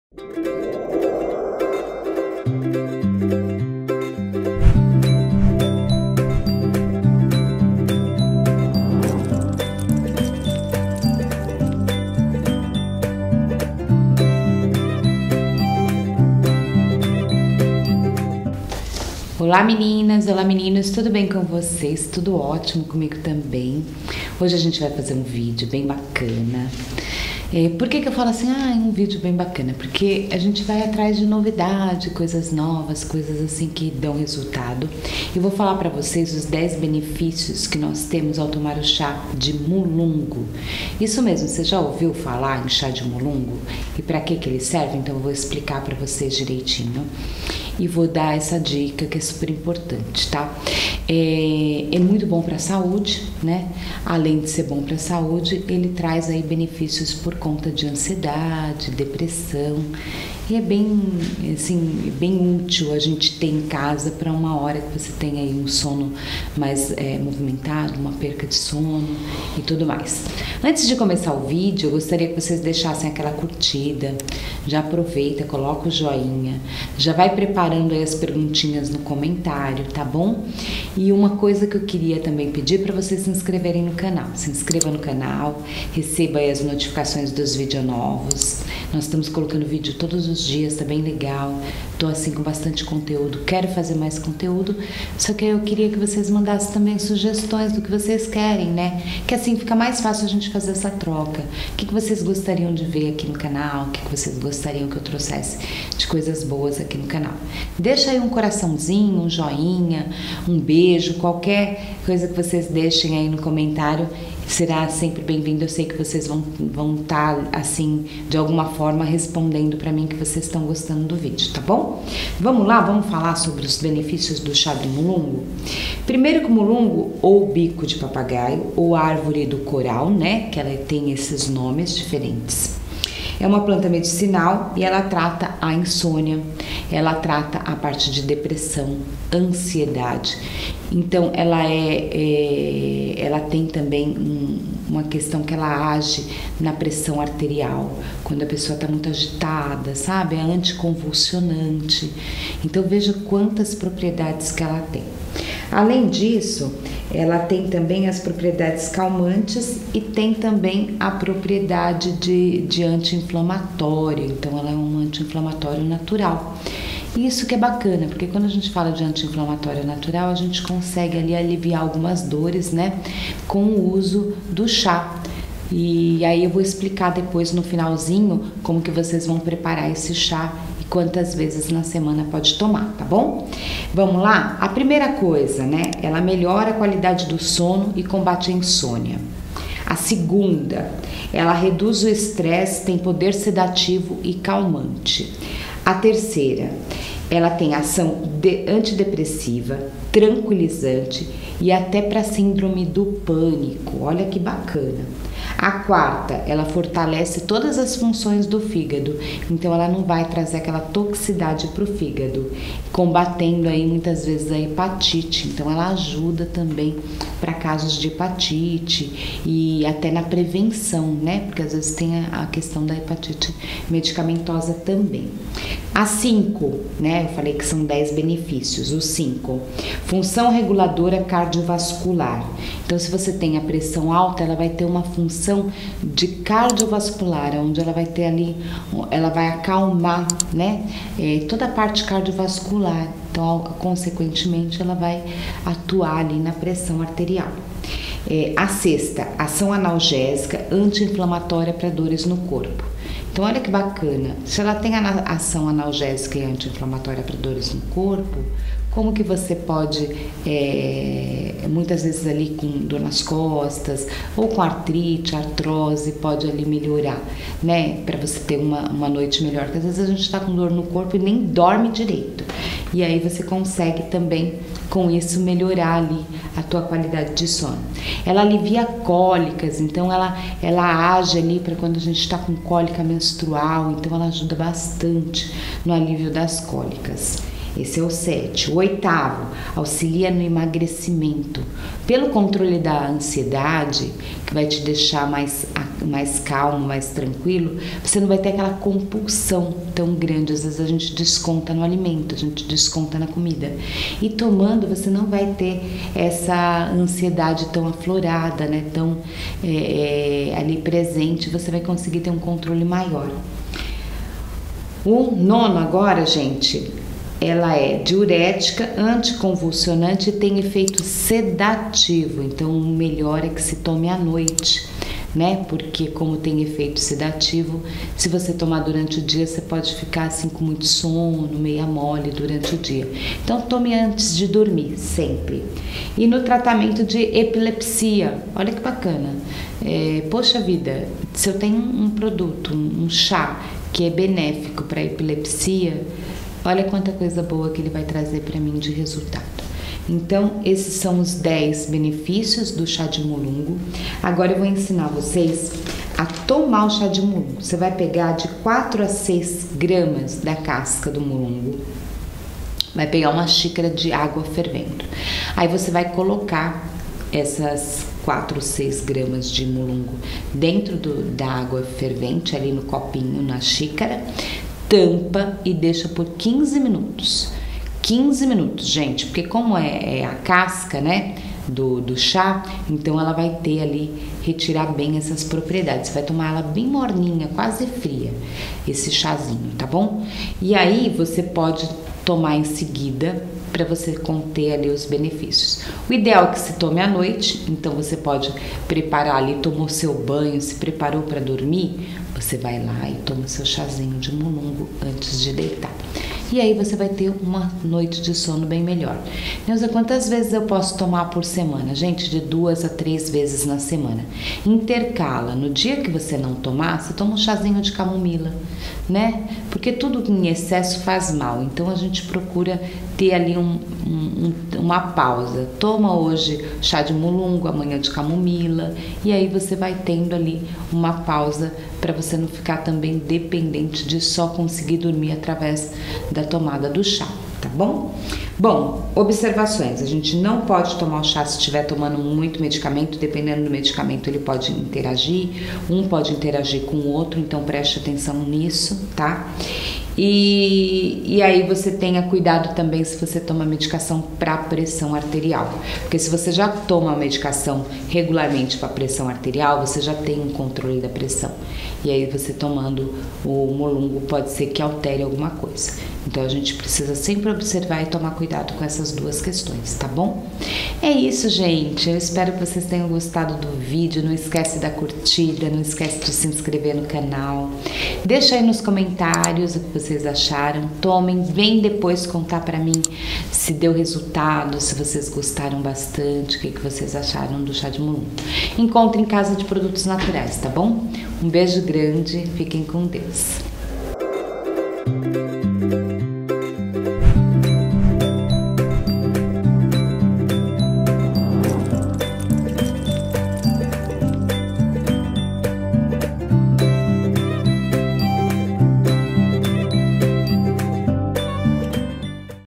10 BENEFÍCIOS do CHÁ de MULUNGU ! UM CALMANTE NATURAL !
Chá de Mulungu excelente para uma boa noite de sono , tranquilizante, calmante, anti inflamatório.

Muitas pessoas com ansiedade e depressão tem se beneficiado desse chá com bons resultados .

❤️ Meu Livro
DEPILAÇÃO PASSO A PASSO

⚠️ As informações disponibilizadas  no canal são de caráter informativo. Não devem ser utilizadas como base para auto-diagnóstico, auto-prescrição ou auto-tratamento. Consulte sempre seu médico .